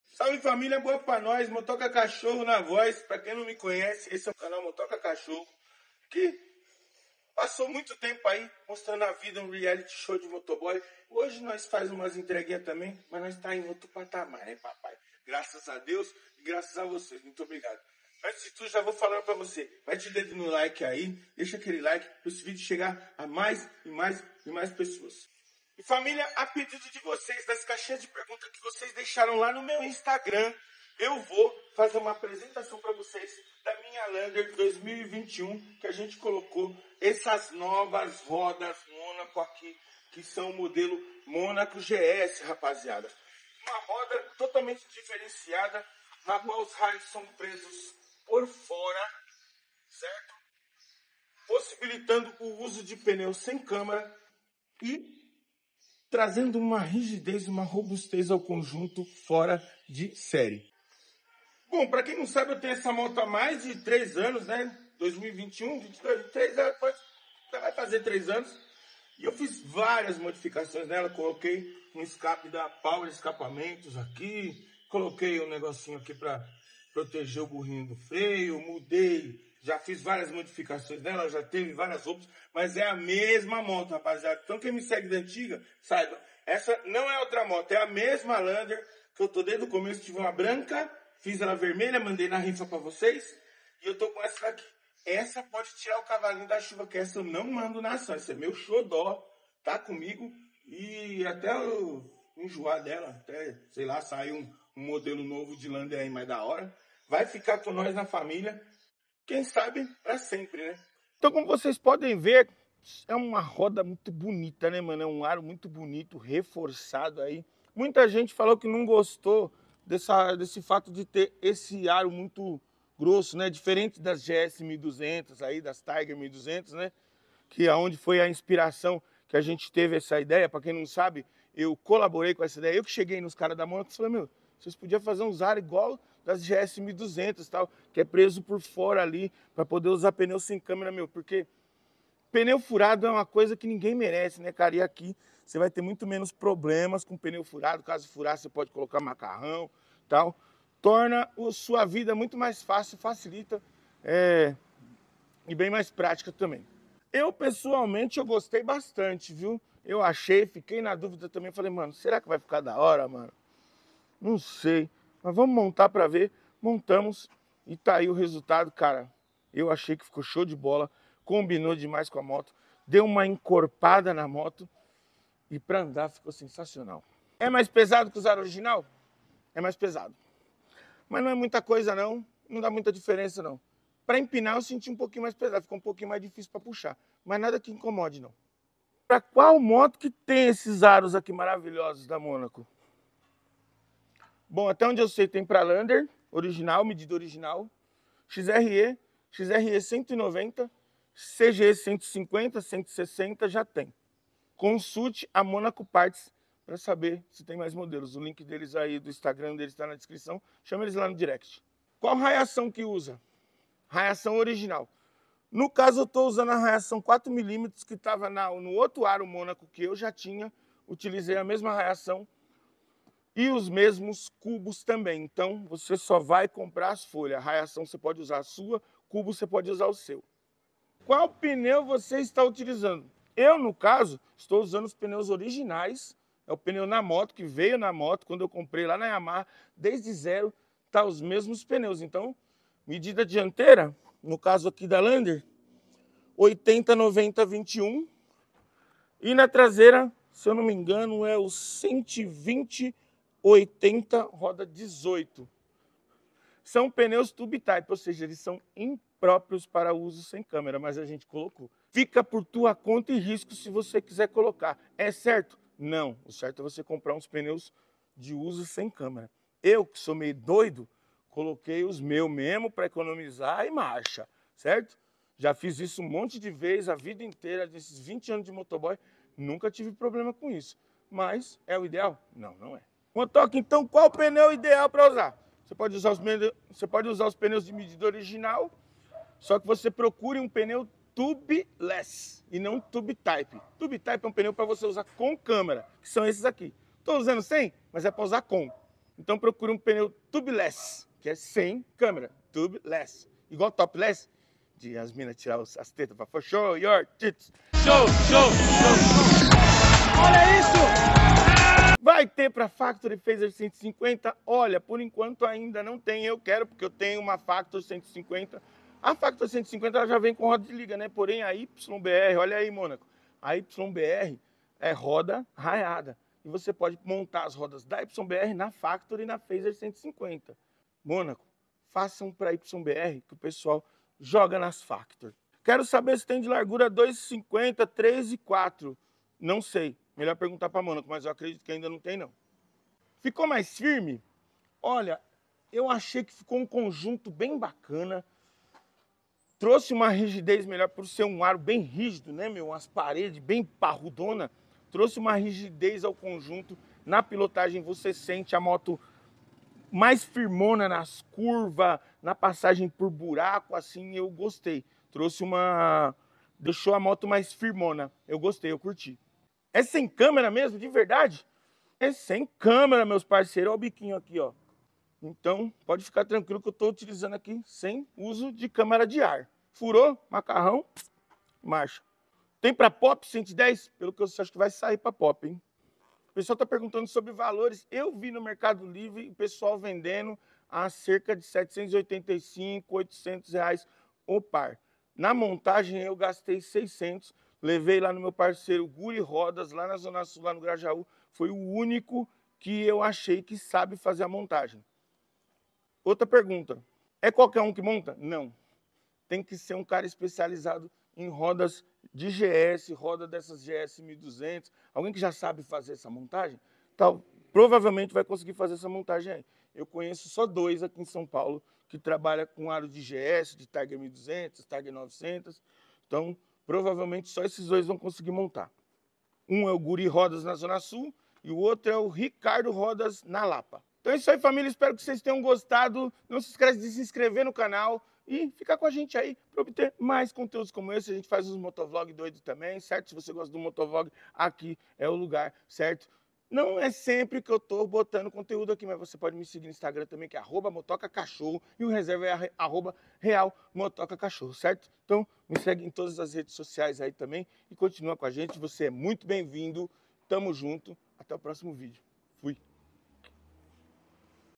LANCEI OS AROS MÔNACO GS NA LANDER NOVA *respondendo dúvidas dos seguidores*
Insta e um dia eu fico no principal e no outro eu fico no reserva.